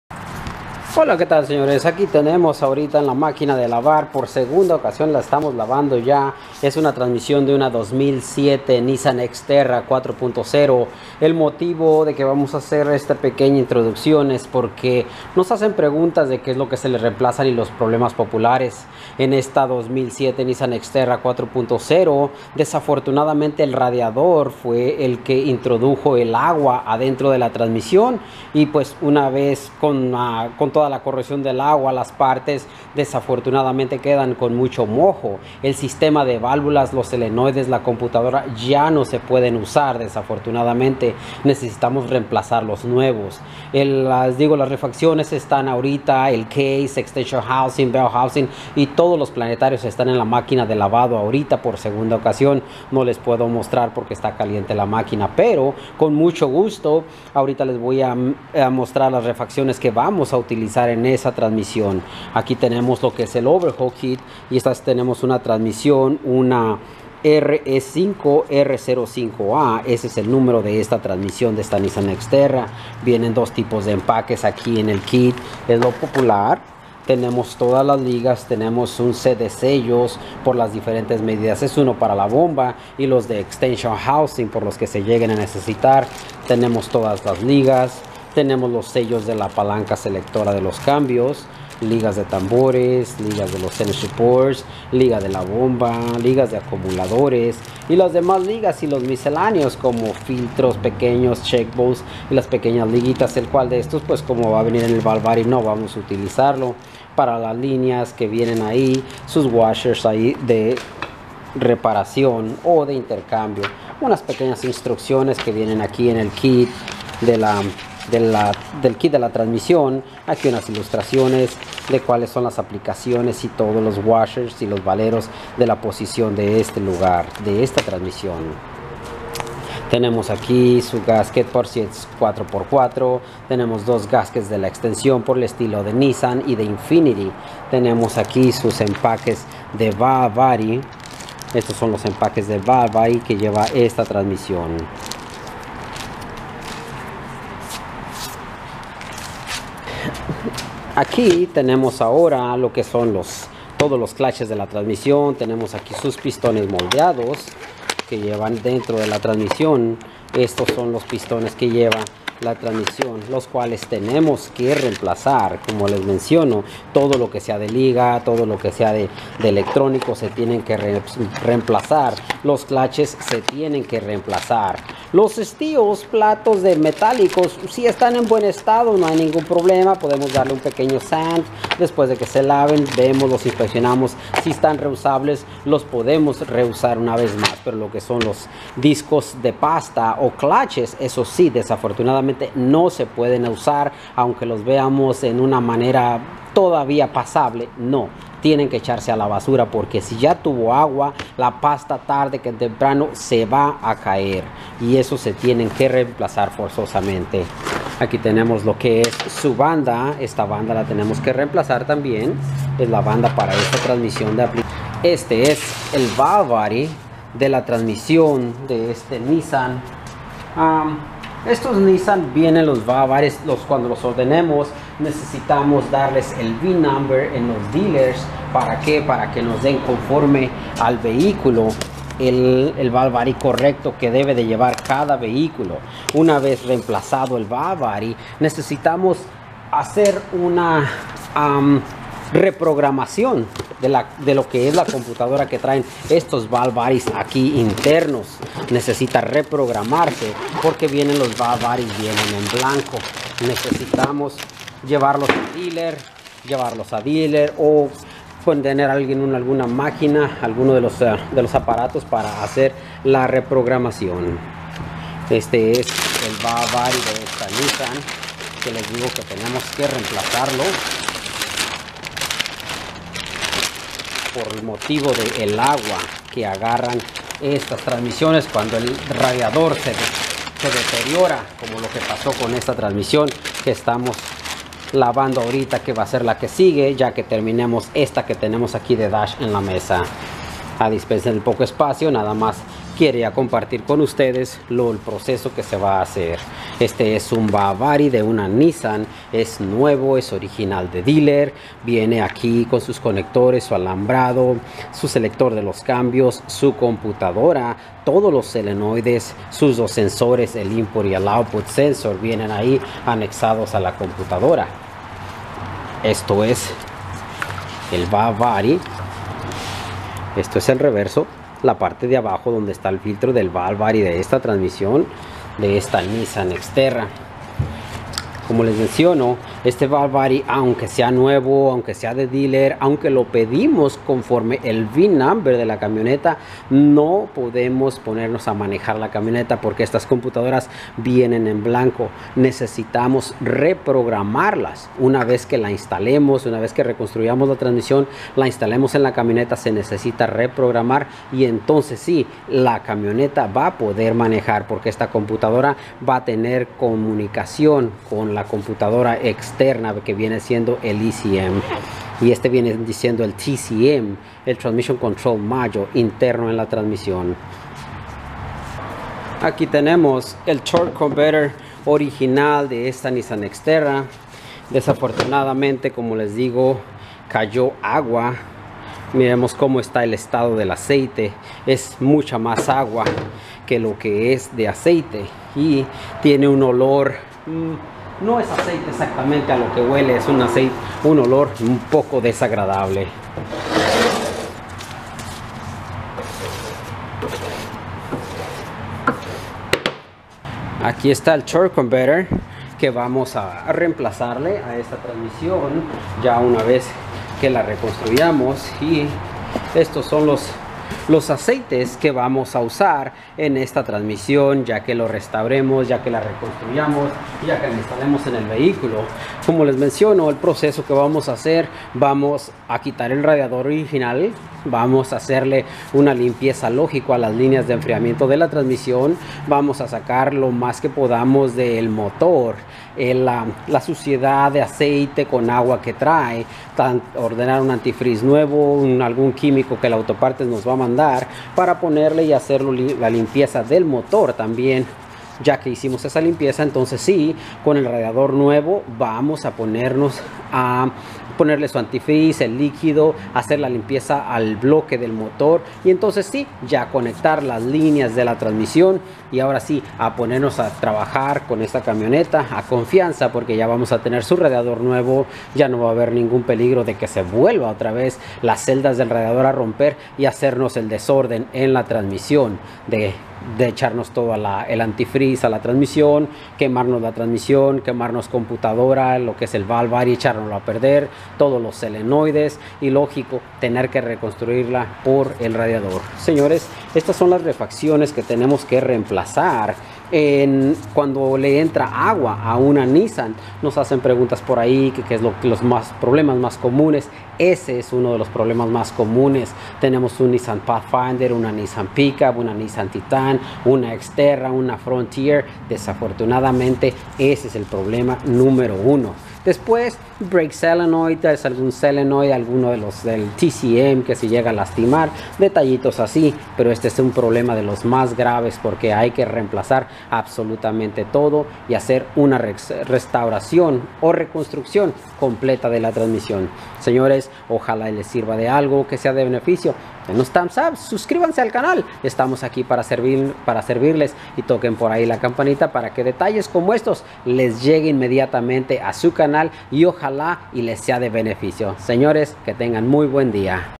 NISSAN TRANSMISSION -REFACCIONES NECESARIAS PARA RECONSTRUIR TU VEHICULO
El proceso y las partes necesarias para reconstruir la transmission de Nissan Xterra, Pathfinder, Titan,Frontier y algunos Infiniti cuando el RADIADOR nos arruina la transmission y todos sus electronicos.